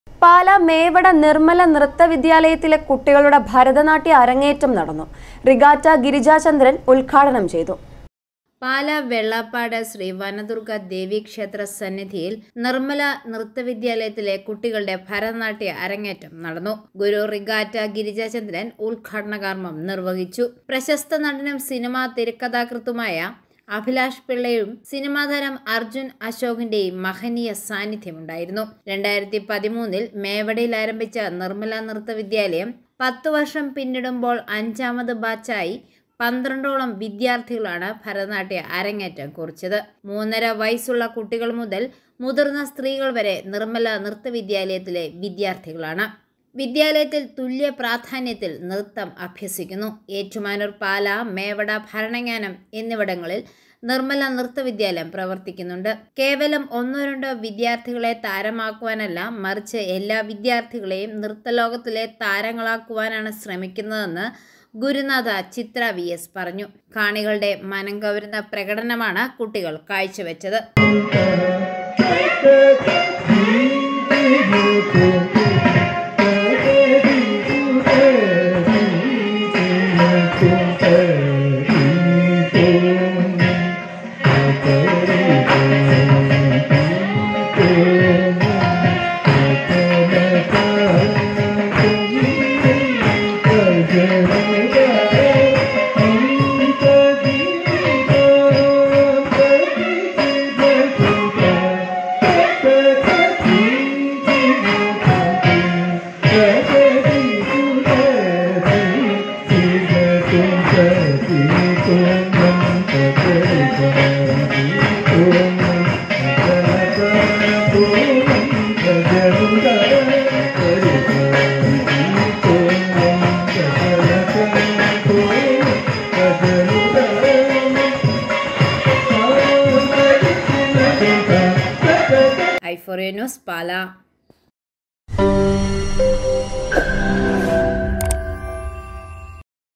പാലാ മേവട നിര്‍മ്മല നൃത്ത വിദ്യാലയത്തിലെ കുട്ടികളുടെ ഭരതനാട്യ അരങ്ങേറ്റം നടന്നു
പാലാ മേവട നിര്‍മ്മല നൃത്ത വിദ്യാലയത്തിലെ കുട്ടികളുടെ ഭരതനാട്യ അരങ്ങേറ്റം നടന്നു. റിഗാറ്റ ഗിരിജാചന്ദ്രന്‍  ഉദ്ഘാടനം ചെയ്തു.
Bharatanatyam debut of the children of Pala Mewada Nirmala Nrytta Vidyalaya was held. The regatta was inaugurated by Girijachandran